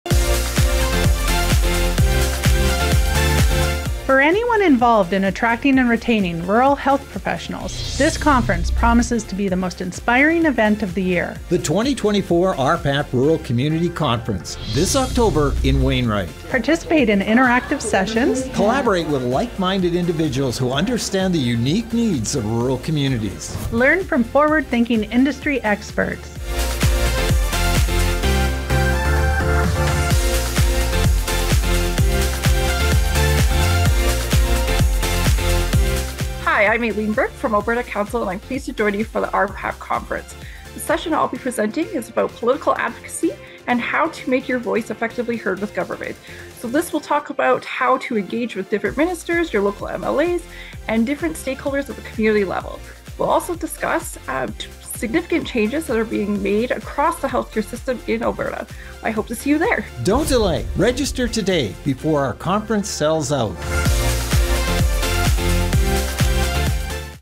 RhPAP Conference Presenter will give tips on how to effectively advocate to government
Aileen Burke is with Alberta Counsel, a team of lawyers and lobbyists.

She will be speaking at the 2024 RhPAP Rural Community Conference in Wainwright, October 8-10.

Burke will talk about political advocacy and how to make your voice effectively heard by all levels of government. She will also discuss the significant changes that are being made across the healthcare system in Alberta.

To find out more about Alberta Counsel, go to albertacounsel.com.